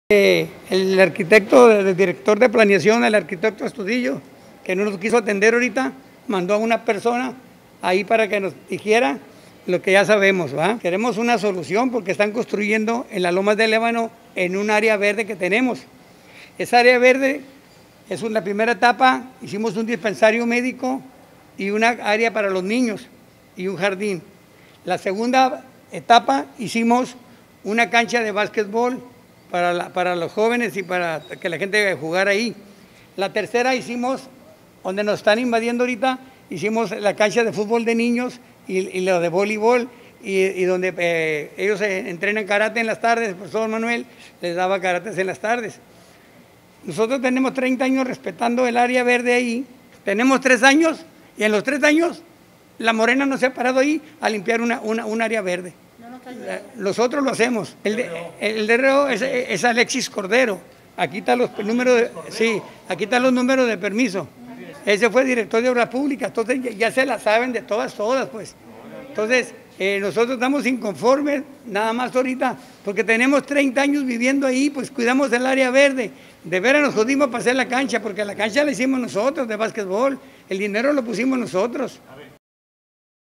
Colonos De Lomas del Ébano Dicen que les “Roban” un Terreno
*Predio destinado a área verde

*Kelly Estavillo y Alexis Cordero